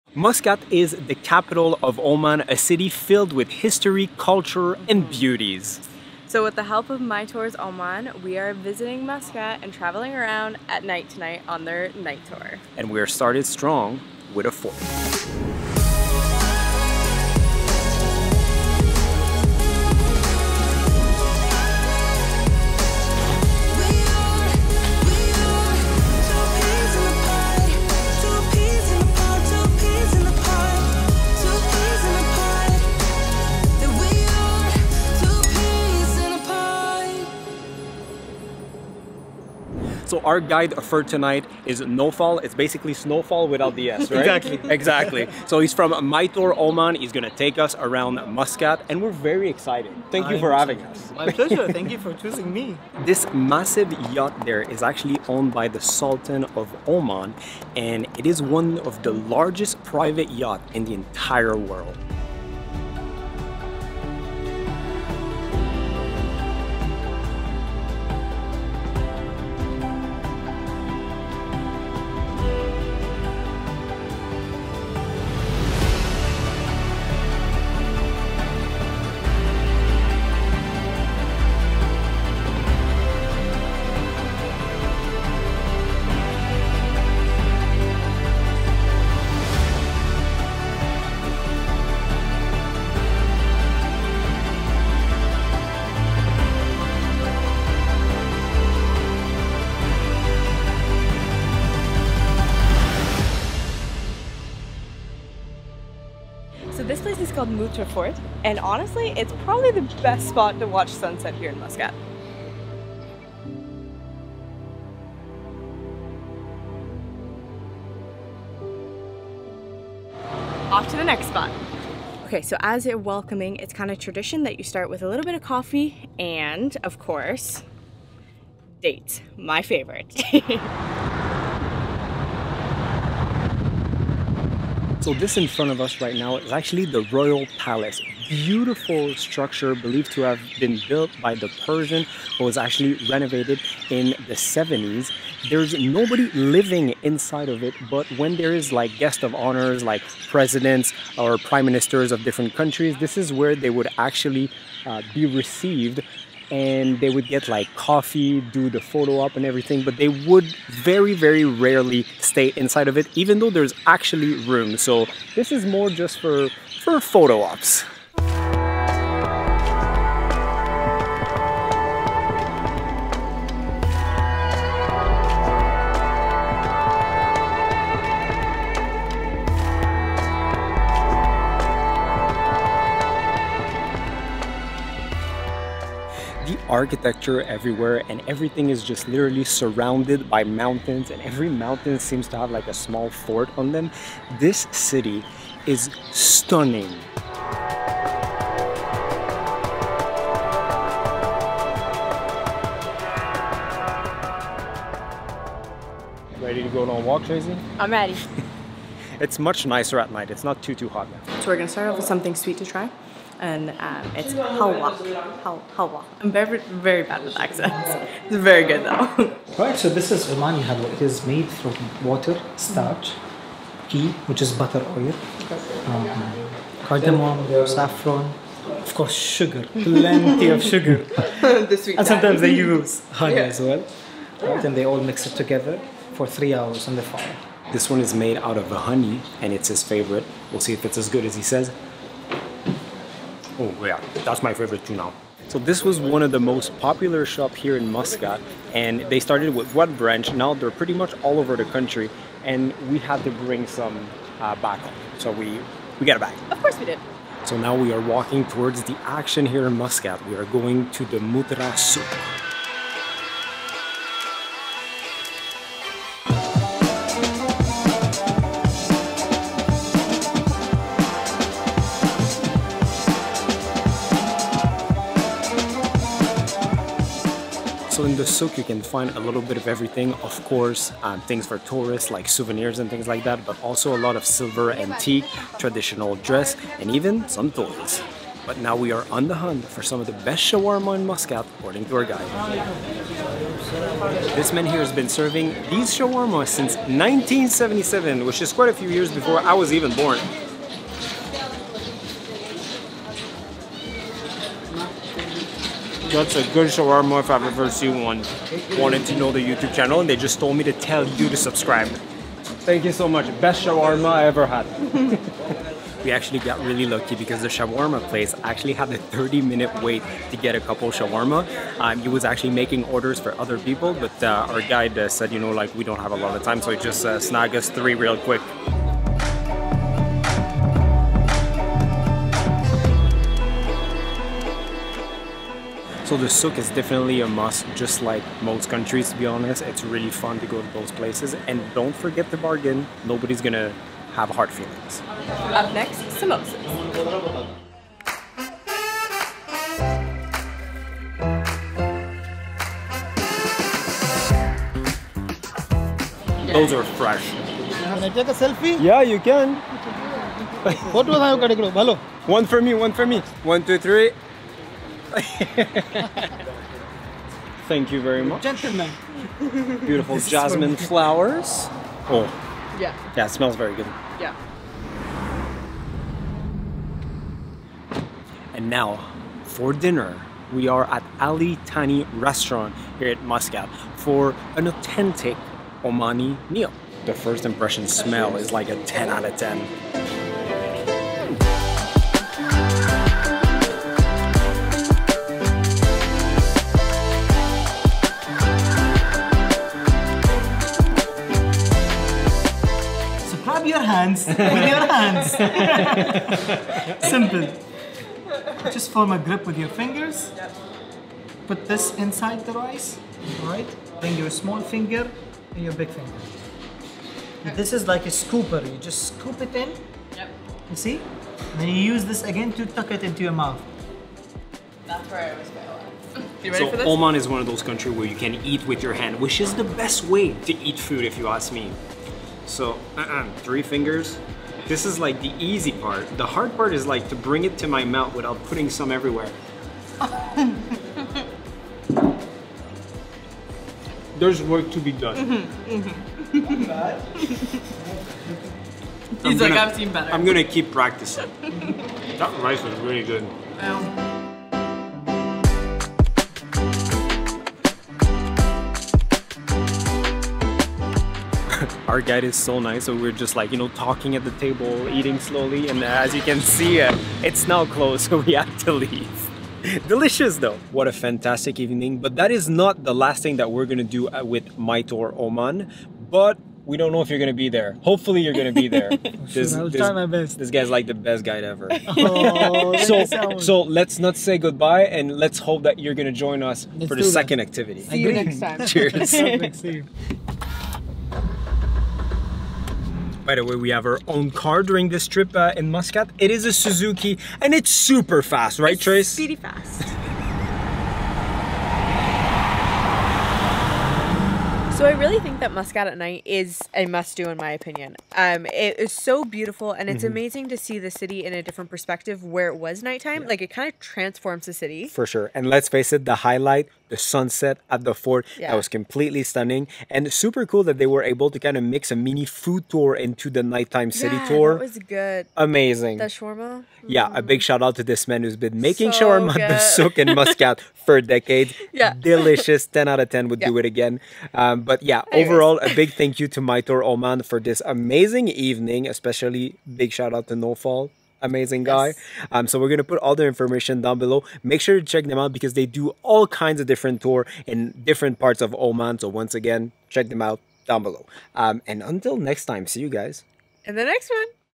Exploring Muscat After Dark / Everything You Need To See & Eat
Muscat is the capital of Oman and it is a city filled with culture, history, entertainment and of course, food! As we always say, there is nothing like visiting a new city with a local and this is exactly what we did with our friends from MyTour Oman.

To make this guided city tour even more interesting, we went for a night time Muscat visit. Keep in mind that most gulf countries, including Oman are so hot during the day that their cities are quiet during the day and very active in the evening.

Our tour started on top of Muttrah at the Muttrah Fort for sunset which was simply stunning! We even had a great view of the Sultan's Yacht, one of the largest privately owed yacht in the world.

We then did a quick stop at the Royal Palace before heading for a walk around the Muttrah Souq. Souqs are always so interesting and this one was no exception. You could find anything from spices, silver, traditional dresses and toys to delicious food and just so much life.

Finally, to end the night, we went for a traditional Omani meal at Ali Thani Restaurant composed of mainly rice, chicken and fish. We truly enjoyed the flavors of Oman.

WANNA SUPPORT US? HERE IS HOW

AMAZON STORE 😍 (Camera Gear & Travel Items)

💸 MAKE MONEY while traveling like us!

Chapters:
0:00 Welcome to Muscat
0:53 Sunset At Muttrah Fort
2:01 Dates & Omani Coffee
2:15 The Royal Palace
3:26 Tasting Omani Sweets
4:17 Walking Around The Muttrah Souq
6:18 Omani Dinner & Learning To Eat With Our Hands
7:51 The Perfect Evening In Muscat